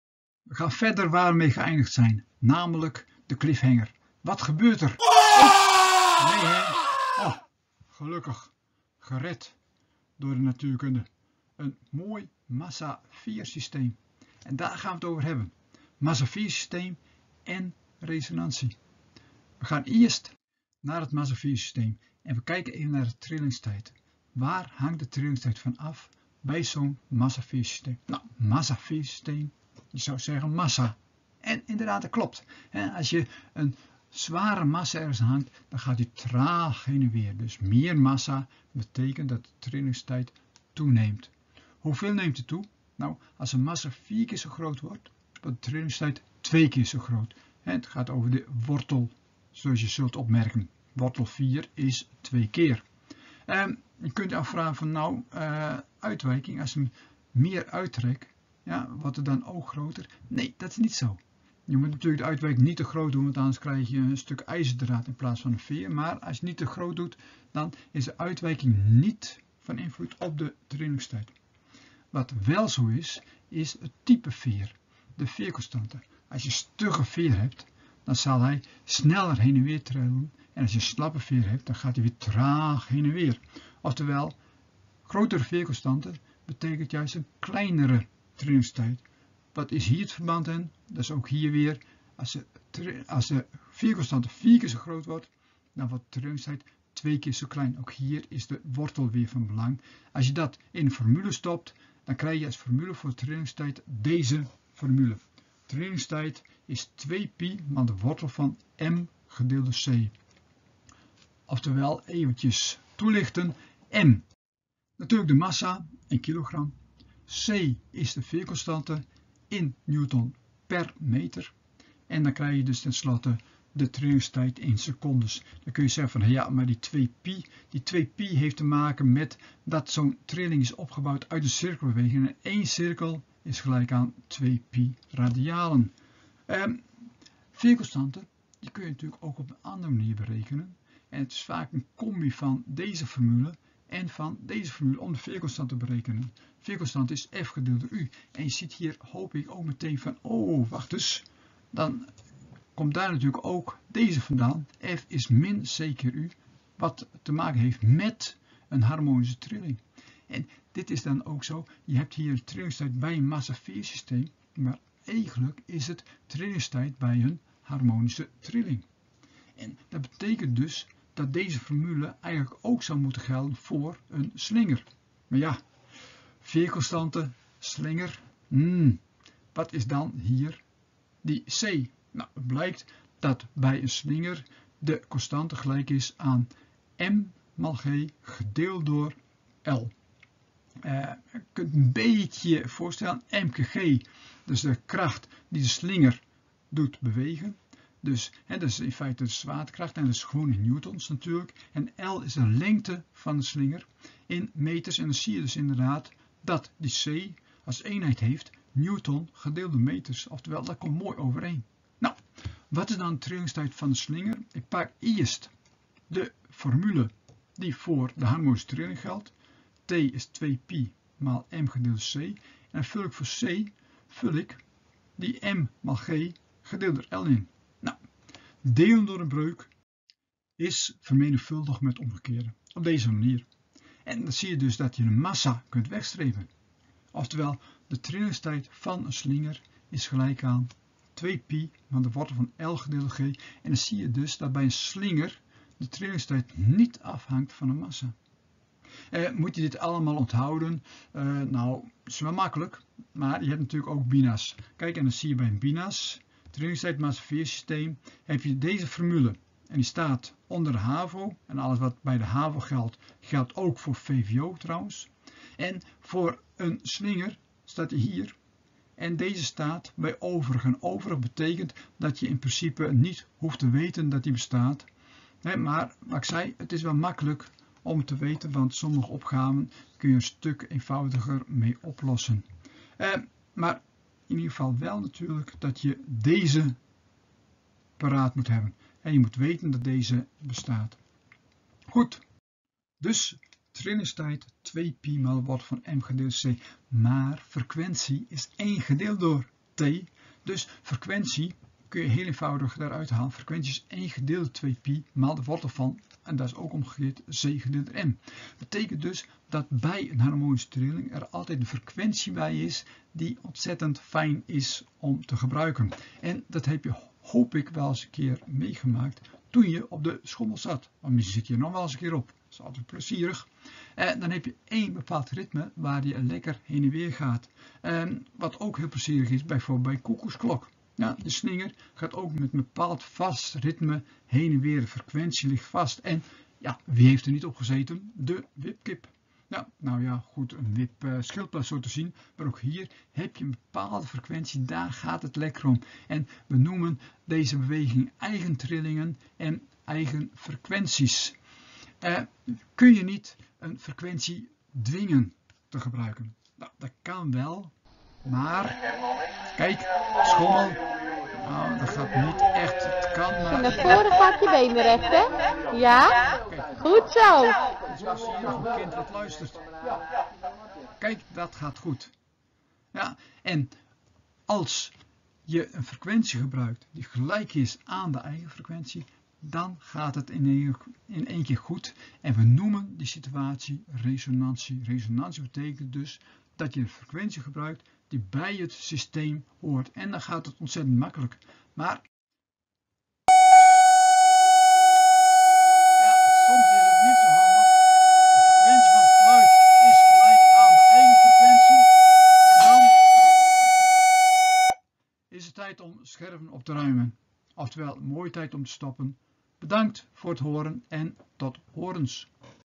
NOVA V5 h07p2 Resonantie
Theorie uit methode NOVA natuurkunde vwo5- hoofdstuk 7, paragraaf 2
- wat is een massa-veer systeem, eigen frequentie en resonantie
- berekeningen kunnen uitvoeren met trillingstijd van een massa-veer systeem